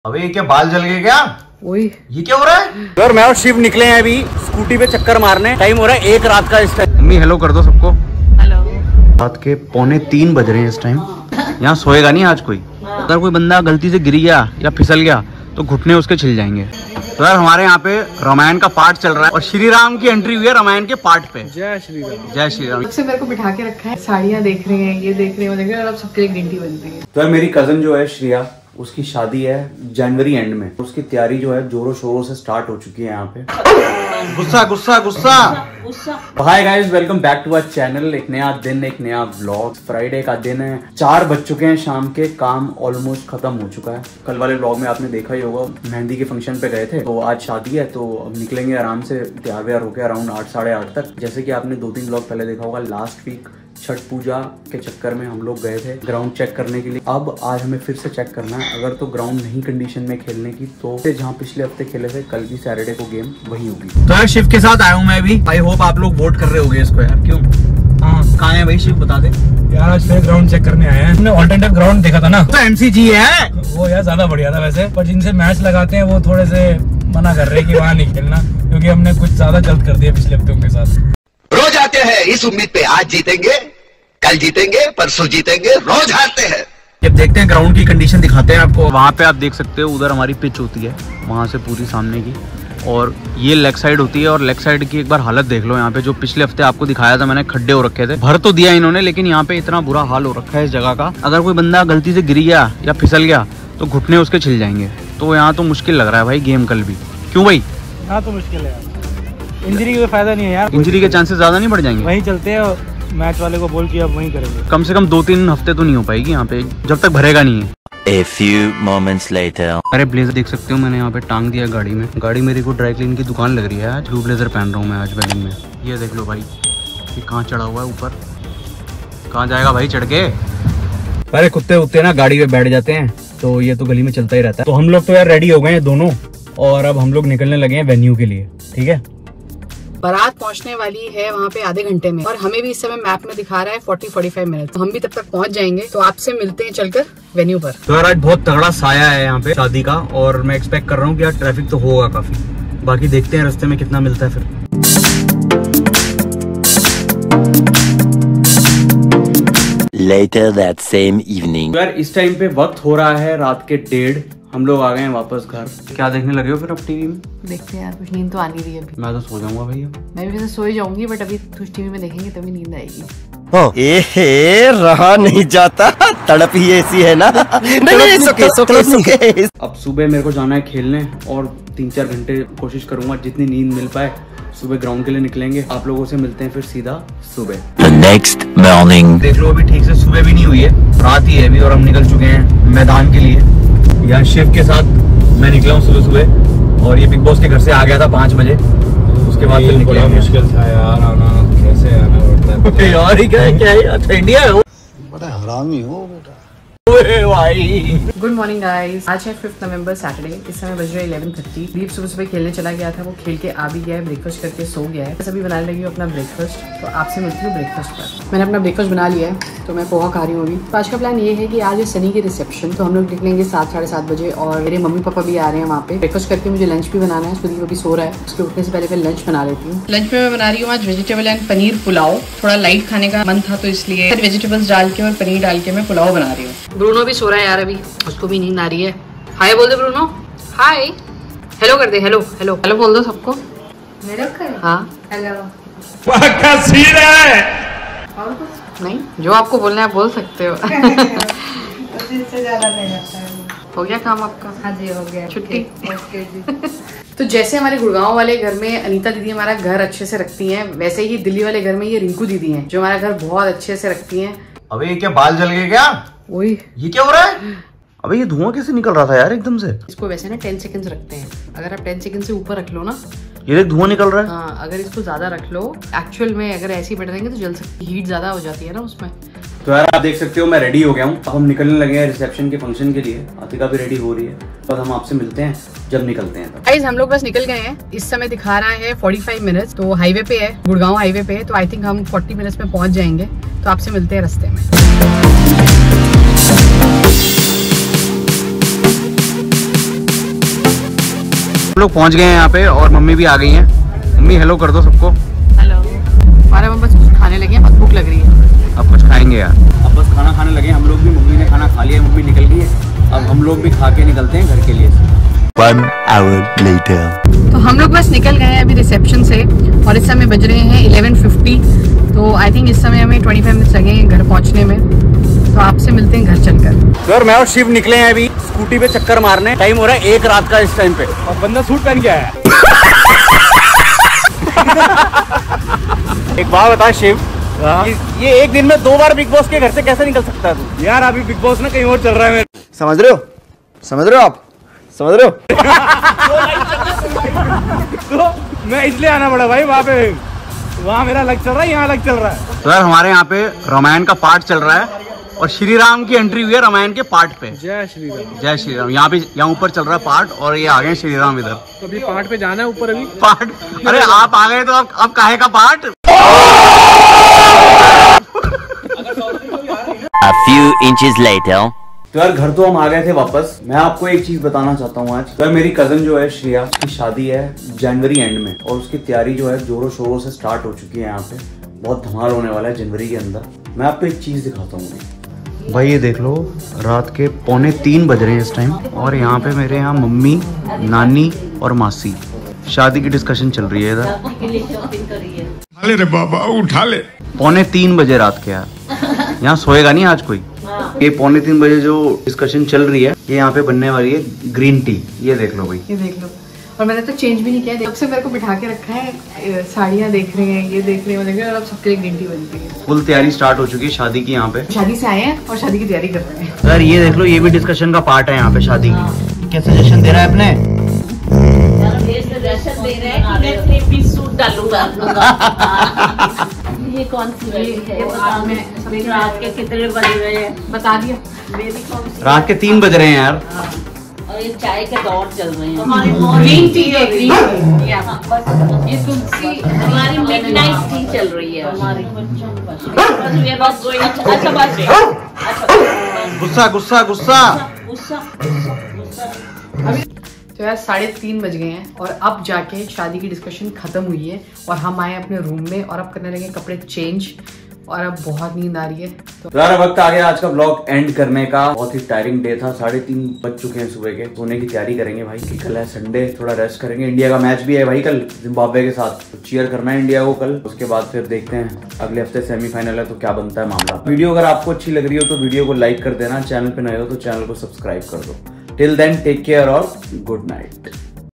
Baal Jal Gaye 😦WTF | Hair On Fire 🔥| Shaadi Ki Tayari Shuru 😍
About Our Channel : Hi we would like to welcome you to our channel.

In this video :

1. Friday begins
2. Ground check.
3. Leaving for wedding.
4. Good night
5. Next day Satuday
6. Sat morning cricket
7. Saturday
8. Leaving for function
9. My cousin wedding preparations start
10. Meeting friends late in the evening
11. Good night.